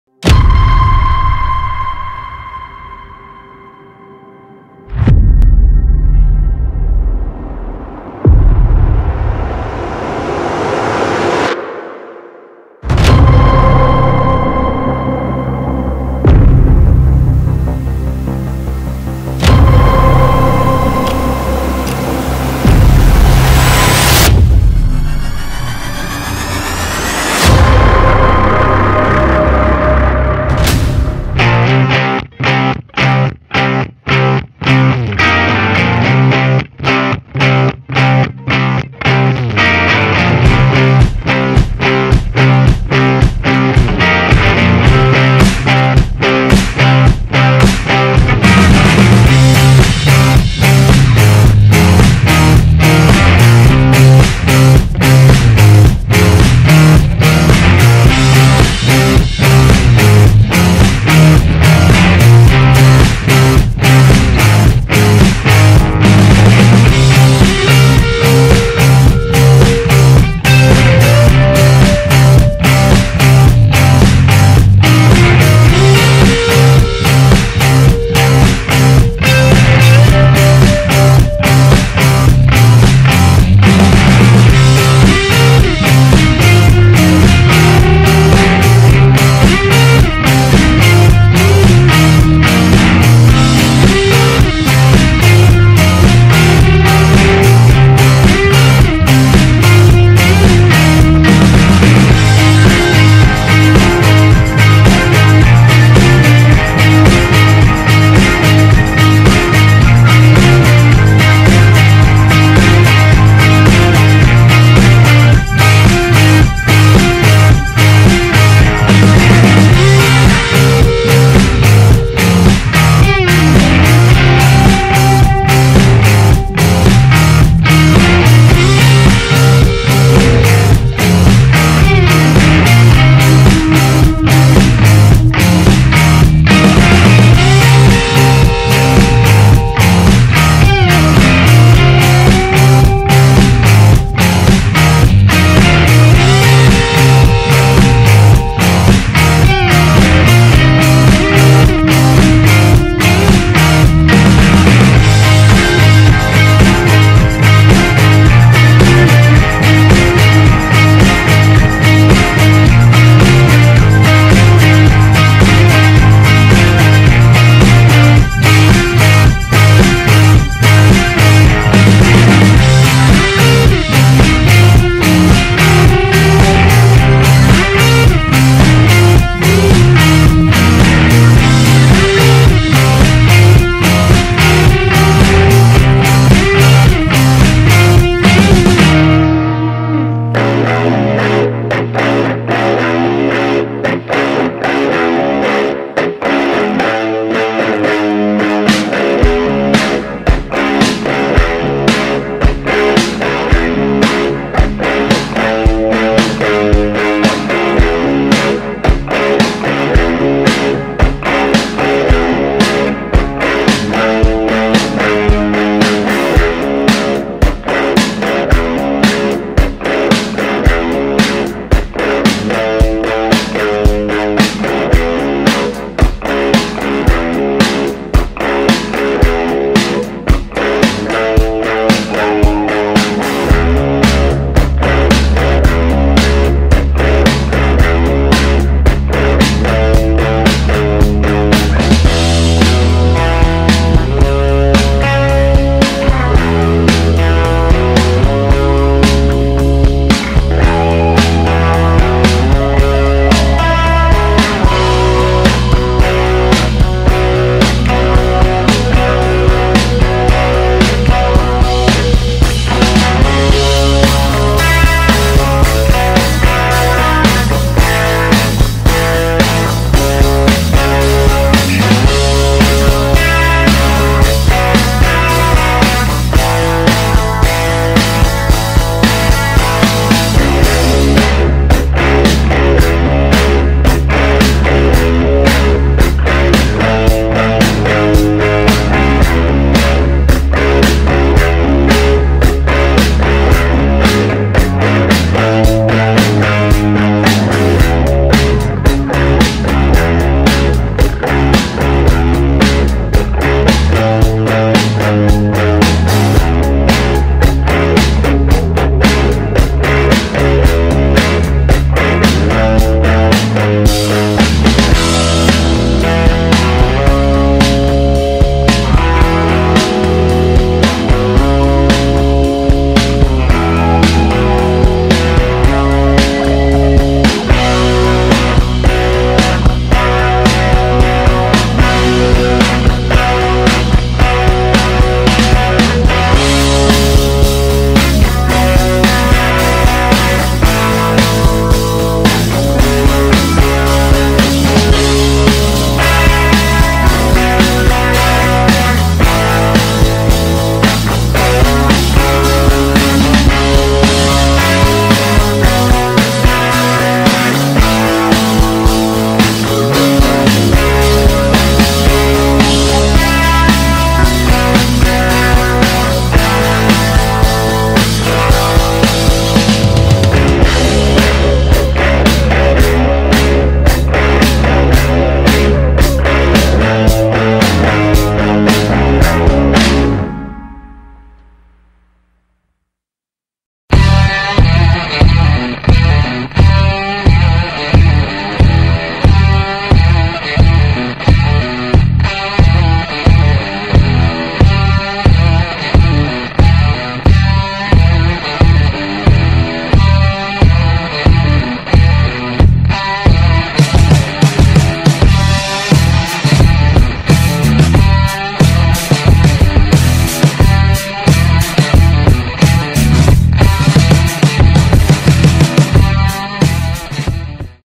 Drawing Sara Zimmerman Duterte-Carpio (aka Inday Sara)
On this episode, i will be drawing Sara Zimmerman Duterte-Carpio commonly known as Inday Sara. Who will be running for the seat of Vice President of the Philippines

You can check my works at these sites!